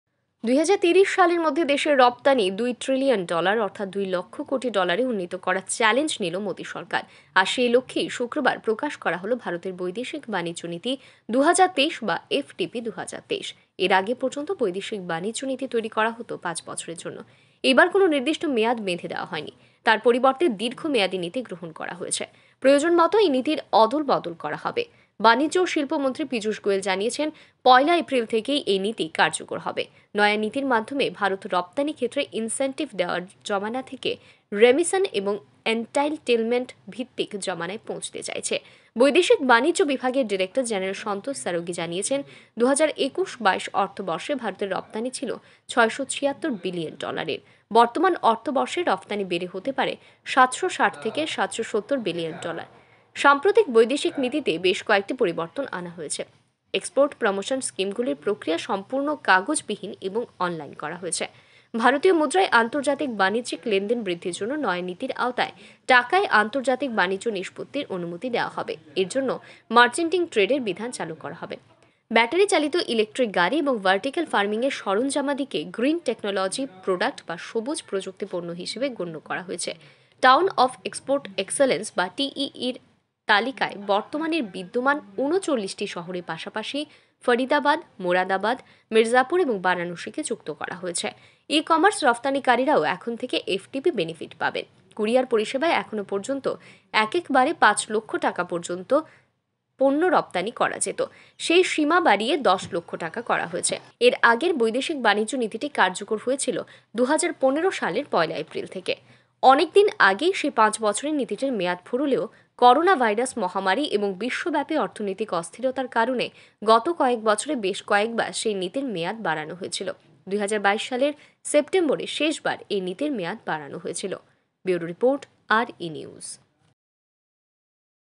৭ বছরের মধ্যে ২ লক্ষ কোটি ডলার রফতানির লক্ষ্যমাত্রা নিল মোদী সরকার
৭ বছরের মধ্যে ২ লক্ষ কোটি ডলার রফতানির লক্ষ্যমাত্রা নিল মোদী সরকার